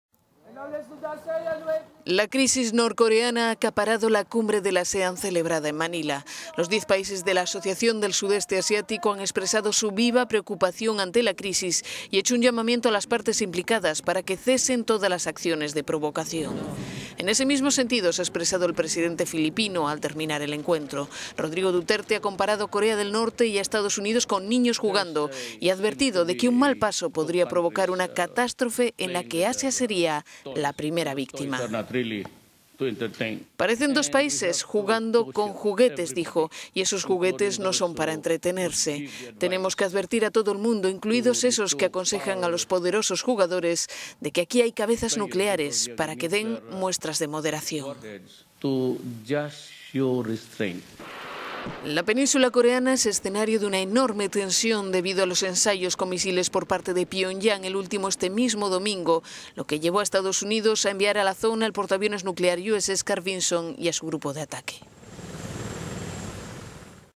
ASEAN pide que cesen la provocaciones en la crisis norcoreana
La crisis norcoreana ha acaparado la cumbre de la ASEAN celebrada en Manila. Los líderes de los diez países de la Asociación del Sudeste Asiático han expresado su "viva preocupación" ante la crisis y hecho un llamamiento a las partes implicadas para que "cesen todas las acciones de provocación".

En ese mismo sentido se ha expresado el presidente filipino al terminar el encuentro. Rodrigo Duterte ha comparado a Corea del Norte y a Estados Unidos con niños jugando y advertido de que un mal paso…